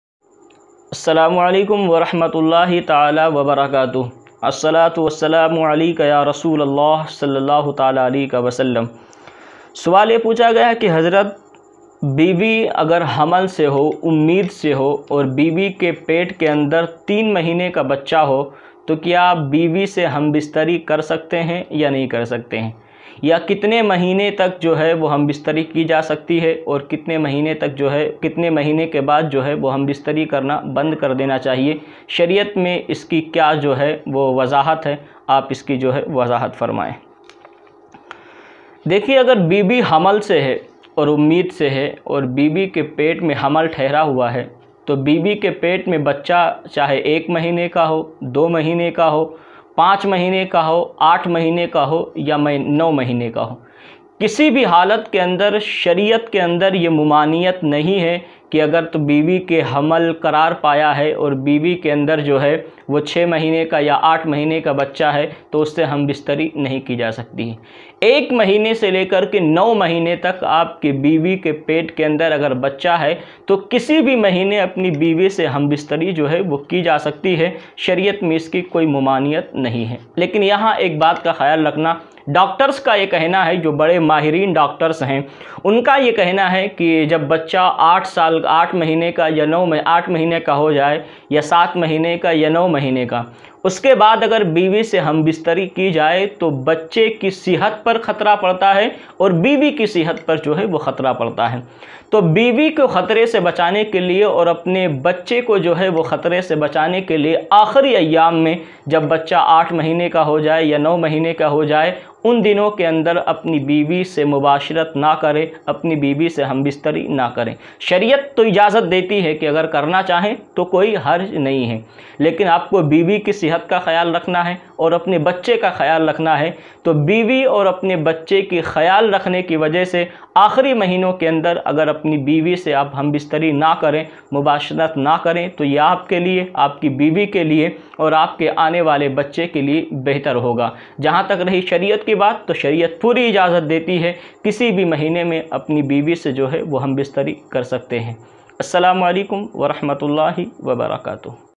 Aurat ke pet me 3 mahine ka bachcha ho to kiya aurat se hambistari kar sakte hain ya nahi
Aaj ke is video me mai aapko batawunga ki auart ke pet me 3 mahine ka bachcha ho to kiya aurat se hambistari kar sakte hain ya nahi ya kitne mahine tak hambistari kar sakte hain aapse guzarish hai ki is video ko pura zaroor dekhe aur ziyadah se ziyadah shere karen